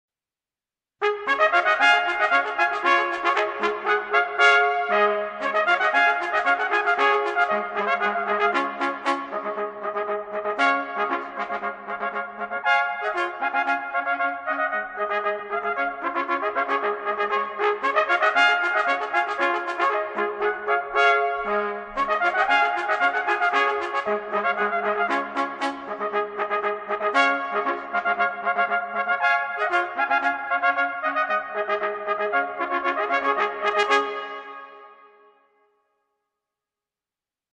UK Baroque Trumpet Ensemble record John Hyde's 1799 trumpet treatise
The University of Kentucky Baroque Trumpet Ensemble has recorded music from the 1799 John Hyde treatise, "A new and compleat preceptor for the trumpet & bugle horn"